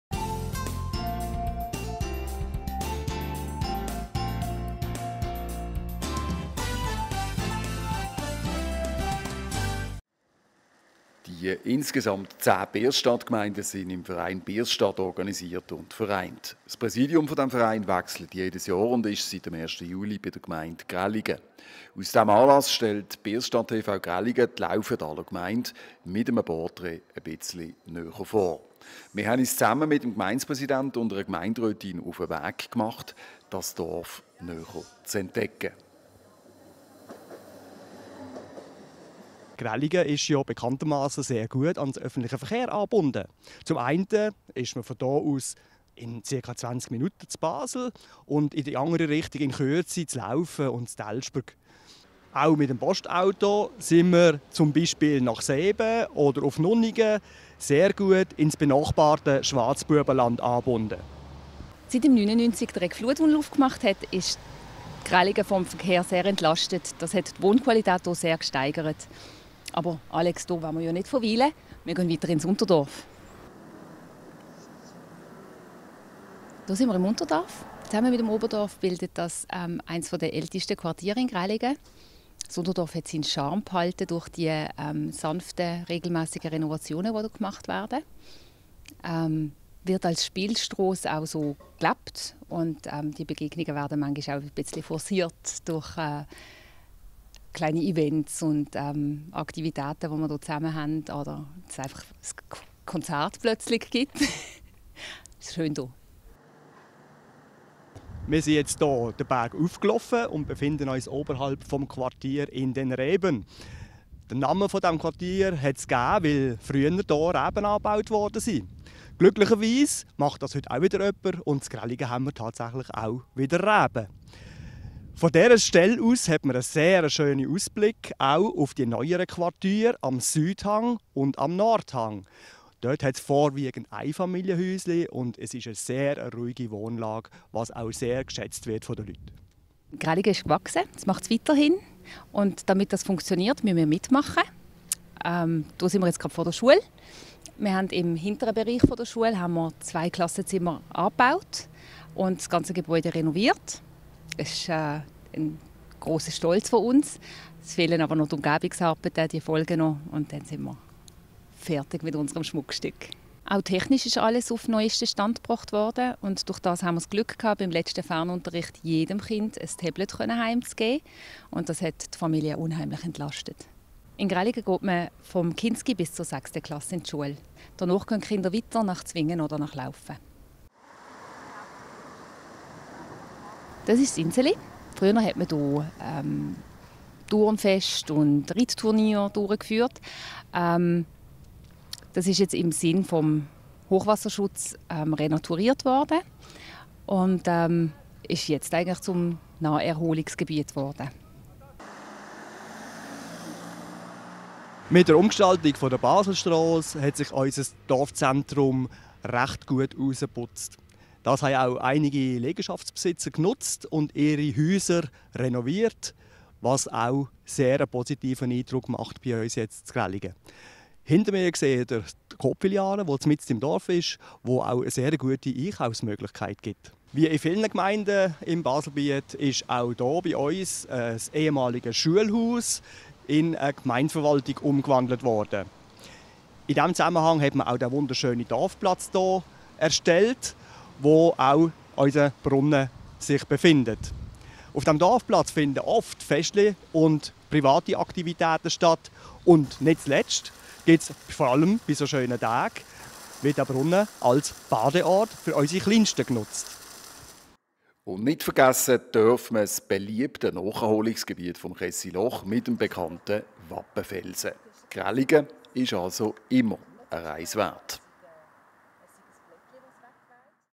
Grellingen im Portrait - Birsstadt-TV Grellingen
Birsstadt-TV Grellingen KW 35: Seit Mitte Jahr präsidiert die Gemeinde Grellingen den Verein Birsstadt. Birsstadt-TV Grellingen nutzt diesen Anlass, um Grellinger etwas näher vorzustellen. Der Film ist eine Entdeckungsreise durchs Dorf mit dem Gemeindepräsidentin Alex Hein und der Gemeinderätin Denise Eichelberger.